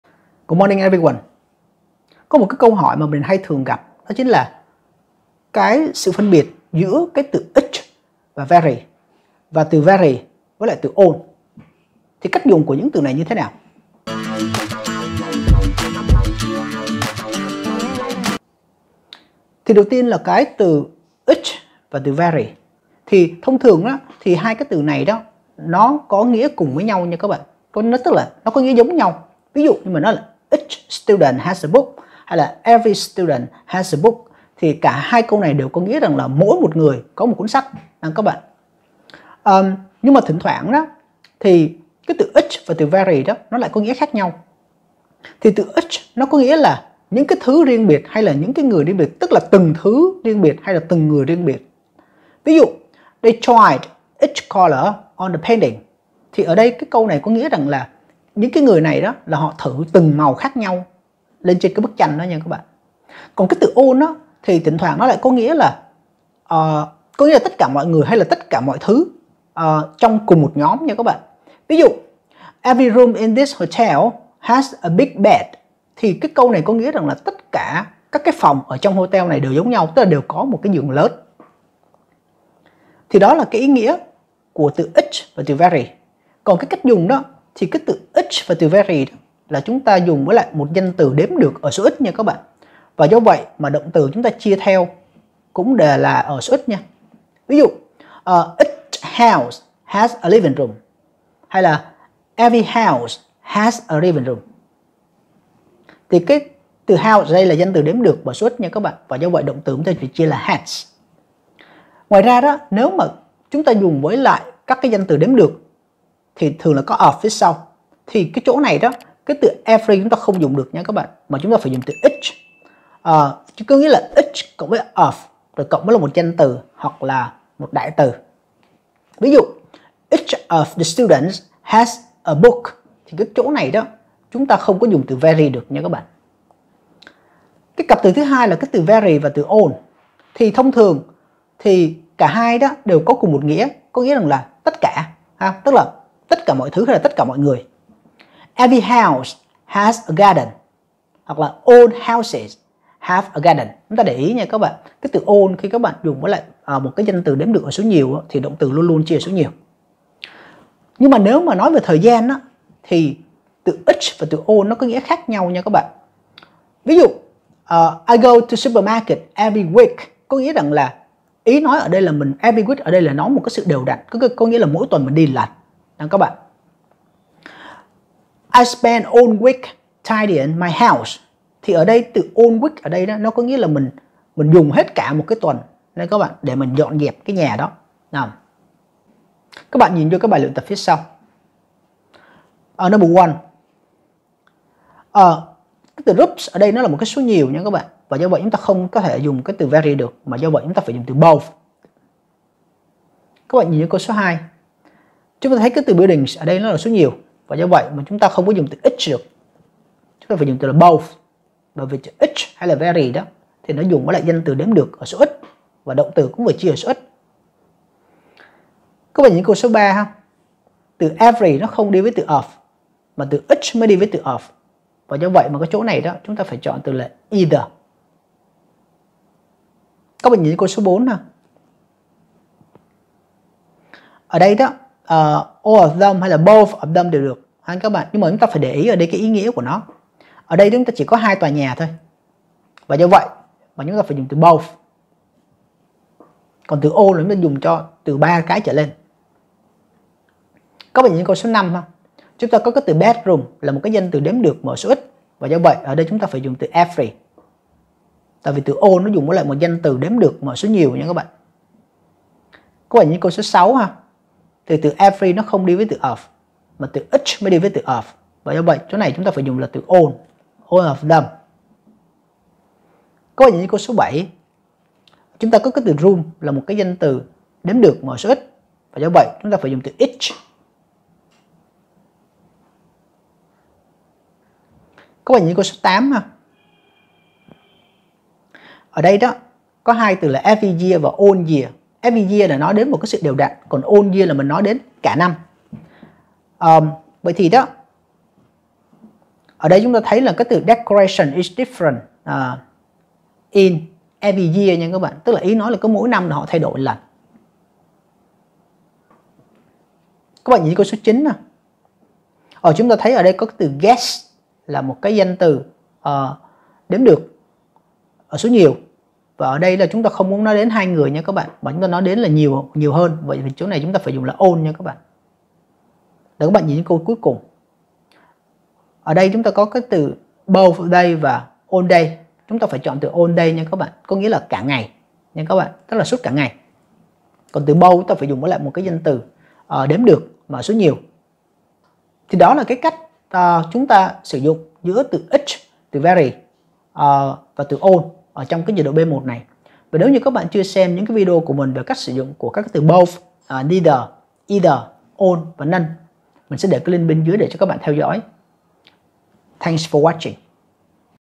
Grammar B1: phân biệt each & every | every & all - Simple English #171
video hôm nay mình sẽ hướng dẫn các bạn cách dùng Grammar B1: phân biệt each & every | every & all nhé
Các bạn muốn tham khảo tài liệu tiếng Anh thì vào